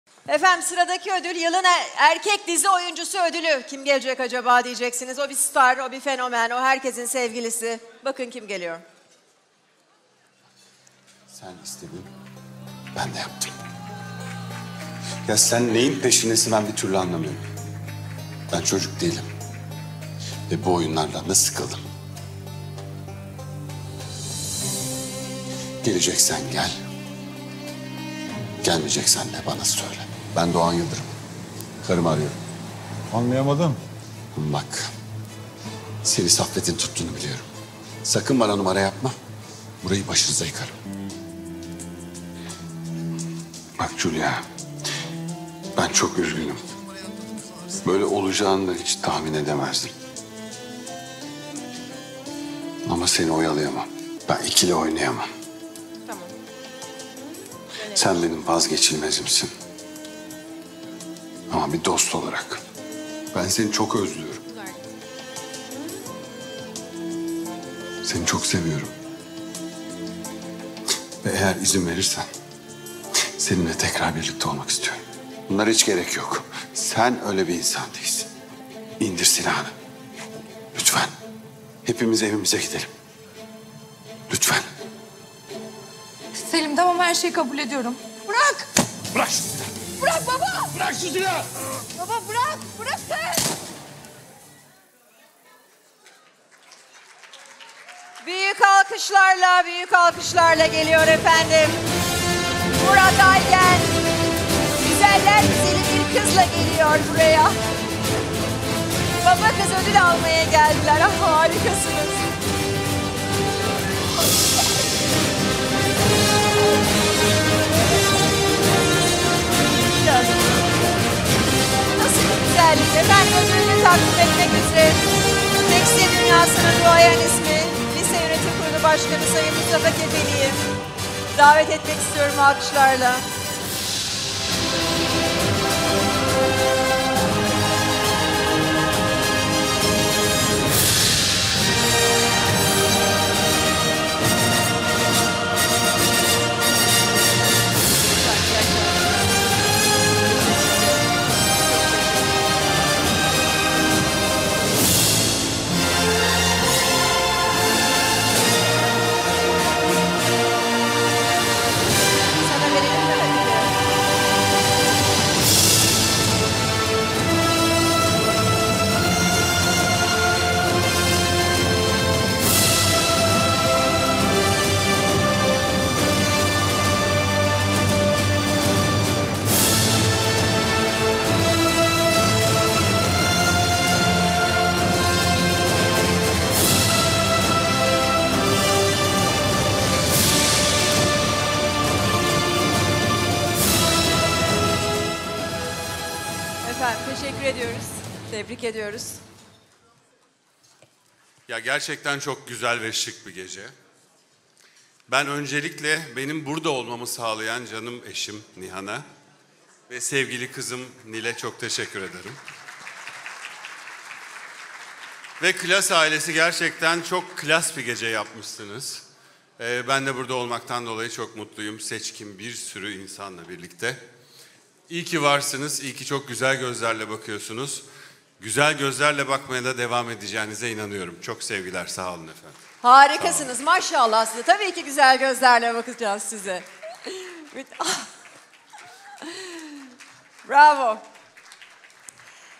MURAT AYGEN (YILIN ERKEK DİZİ OYUNCUSU)
Yasak Elma dizisinde sergilemiş olduğu performansıyla taraflı tarafsız herkesi kendisine hayran bırakan ve büyük beğeni toplayan Murat Aygen Yılın Erkek Dizi Oyuncusu ödülünü almaya hak kazandı.